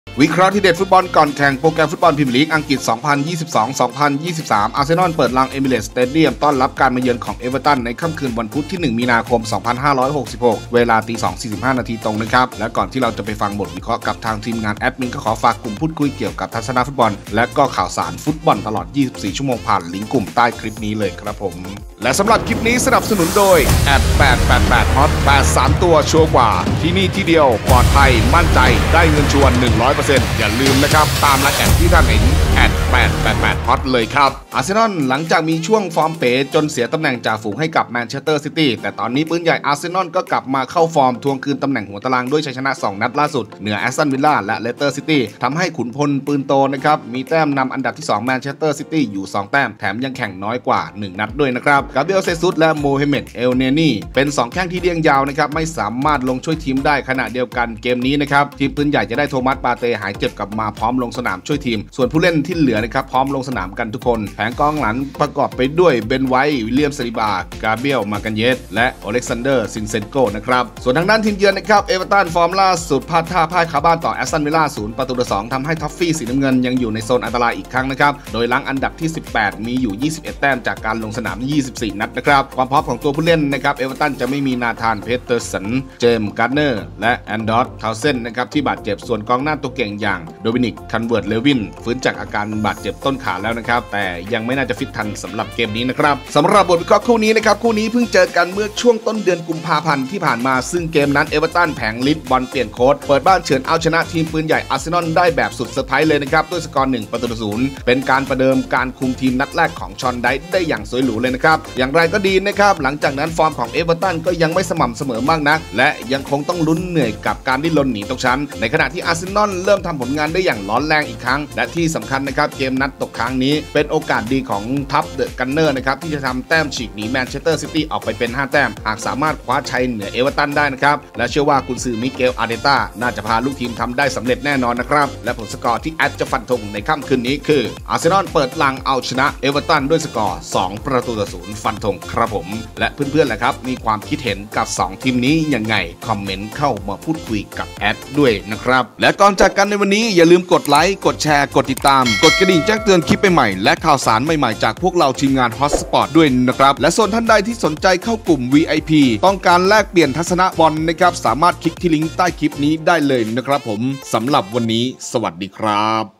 วิเคราะห์ฟุตบอลวันนี้ l 01.03.66 l อาร์เซน่อล vs เอฟเวอร์ตัน l พรีเมียร์ลีกอังกฤษ (Premier League)
โปรแกรมบอลวันนี้ l วิเคราะห์ฟุตบอลวันนี้ l 01.03.66 l อาร์เซน่อล vs เอฟเวอร์ตัน l พรีเมียร์ลีกอังกฤษ (Premier League)
00:00  รายการ : ฟุตบอลพรีเมียร์ลีก 2022/23
แมตช์การแข่งขัน : อาร์เซน่อล พบ เอฟเวอร์ตัน
วัน/เวลาแข่งขัน : คืนวันพุธที่ 1 มีนาคม 2566 เวลา 02.45 น.
สนาม : เอมิเรตส์ สเตเดี้ยม

📲ดูบอลสดลีกดังได้ที่นี่ 👇
ขอบคุณข้อมูลจาก trueID visions , trueID sport , true visions community church